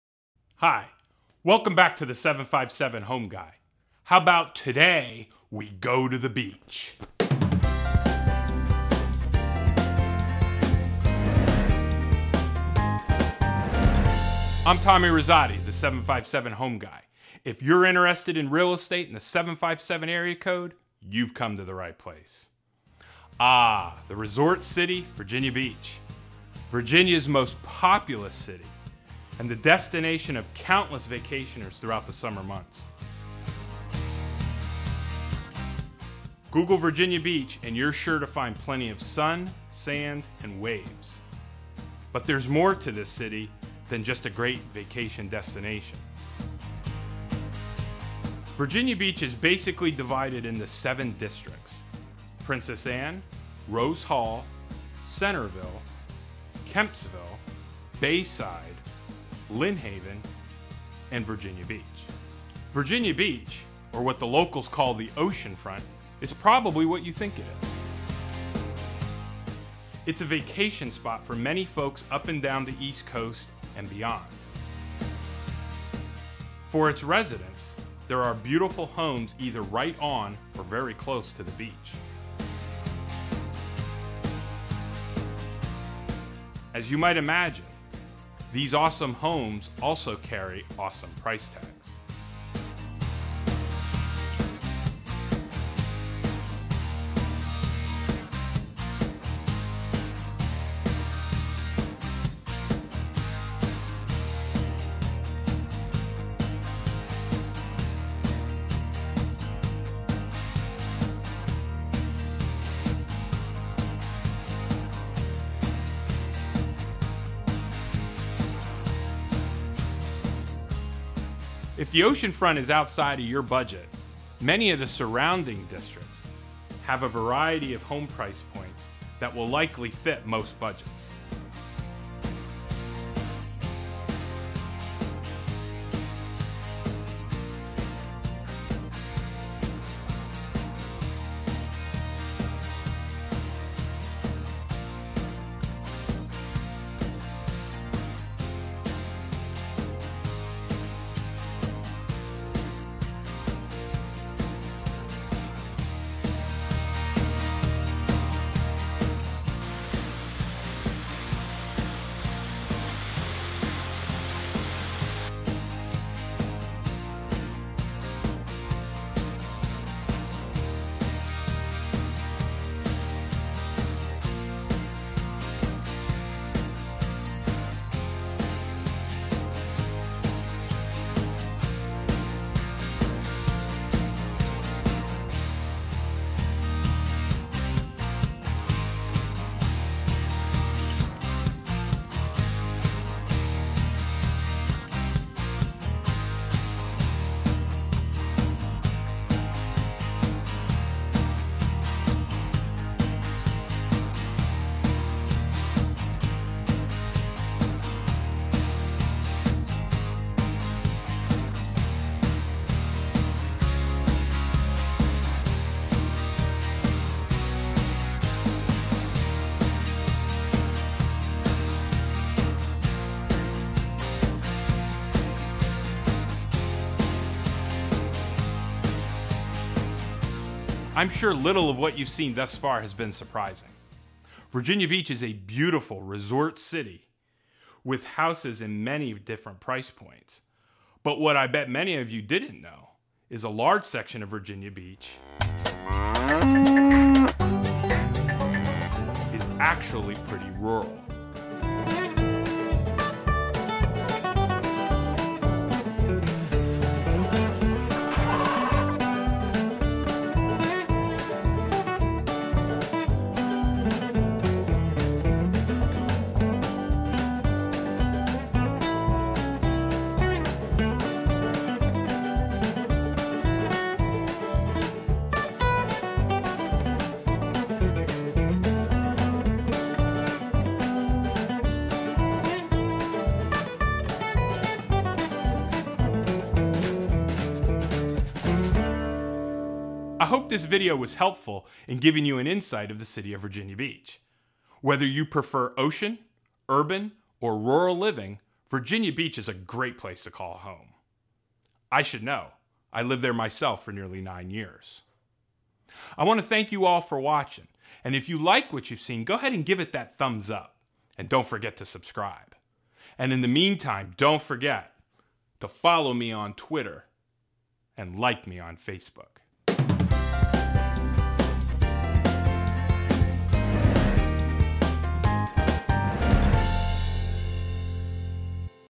The City of Virginia Beach, Virginia
If you're thinking about buying a home in Virginia Beach Virginia, you need to watch this video!  Tommy takes you through the major sections of the city and display's current home prices in each.